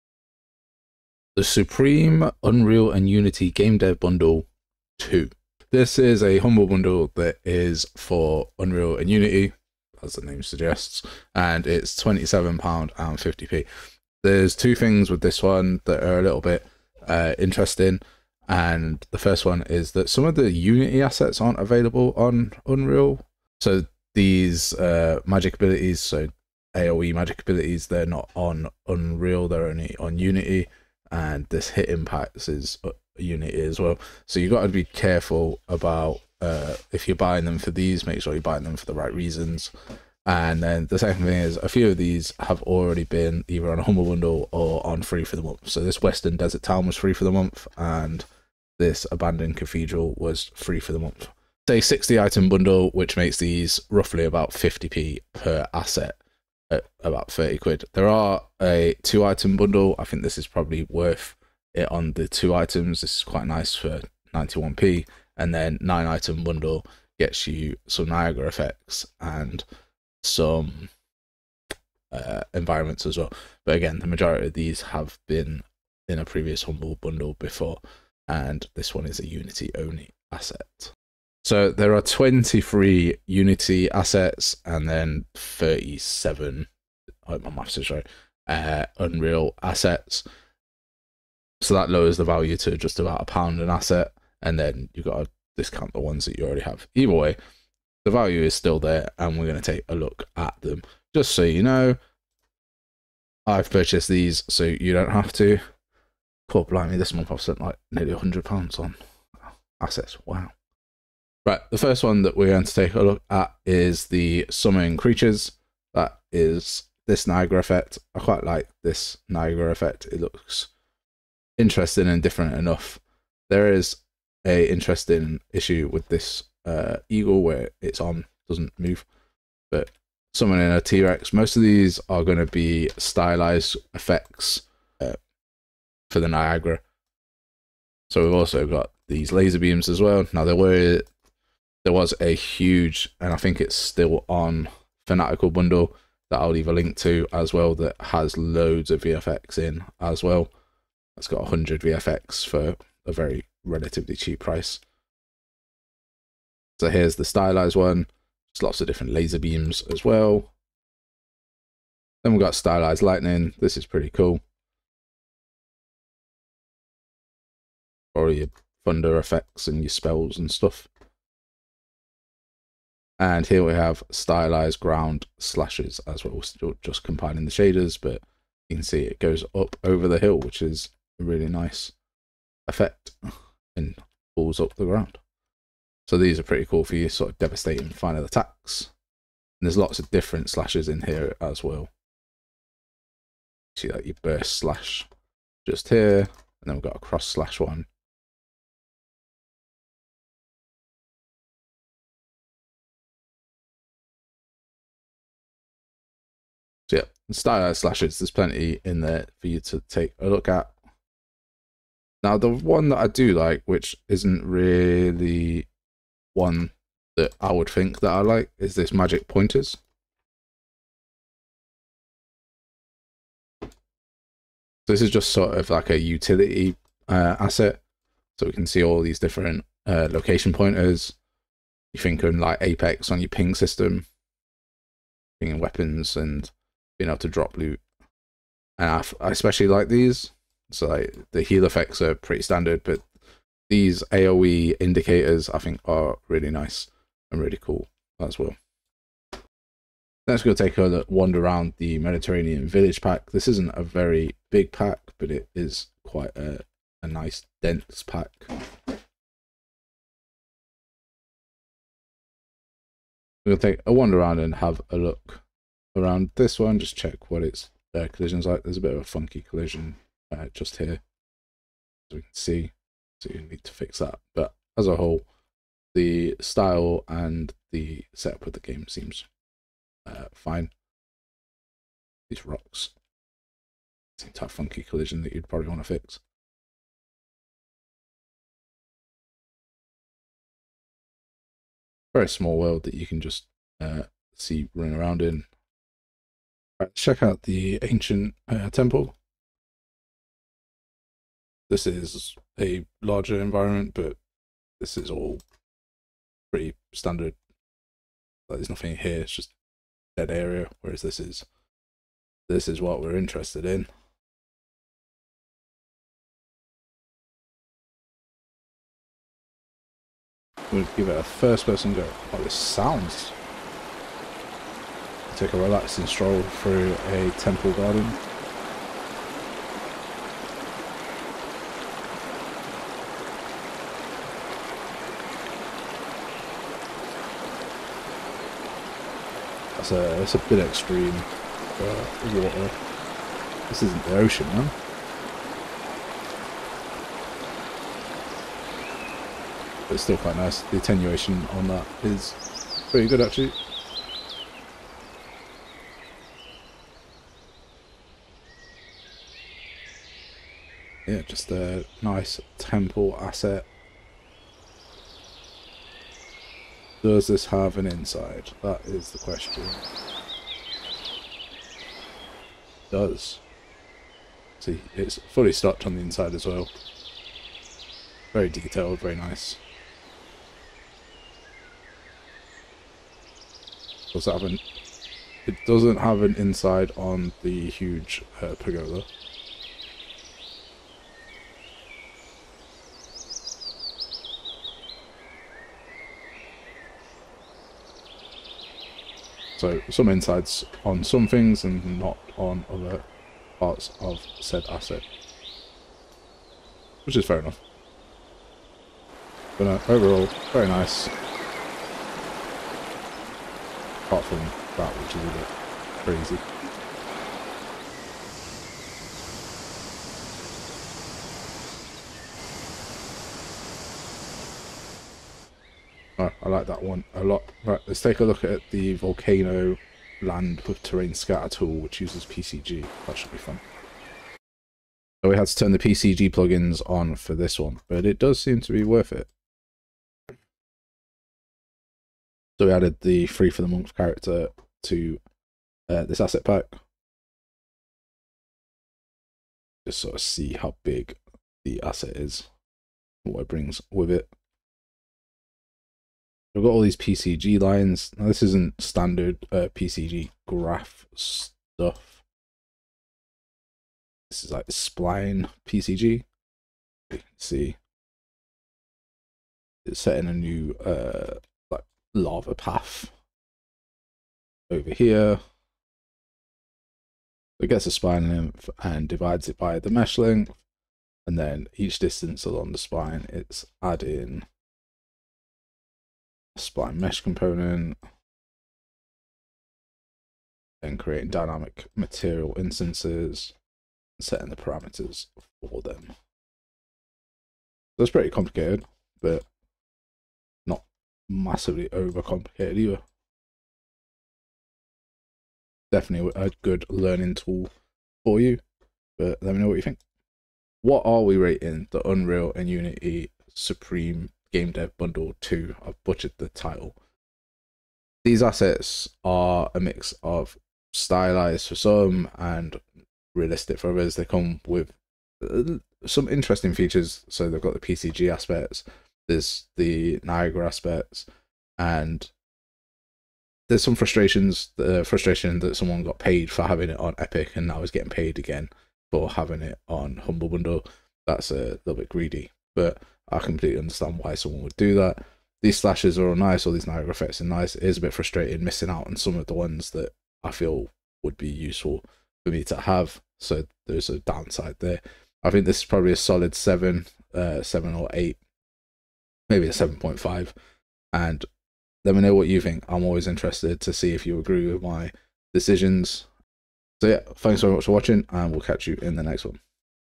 The Supreme Unreal & Unity Game Dev Bundle 2 Unreal Engine Humble Bundle Review
The Supreme Unreal & Unity Game Dev Bundle 2 Unreal Engine Humble Bundle Review, we look at the bundle and check what the real value is.

00:00 - 02:00 - intro
02:00 - 04:00 niagara vfx showcase
04:00 - 05:00 meditarnian village
05:00 - 07:00 Temple
07:00 - 08:30 volcano pcg
08:30 -09:30 final verdict

special thanks to my paid members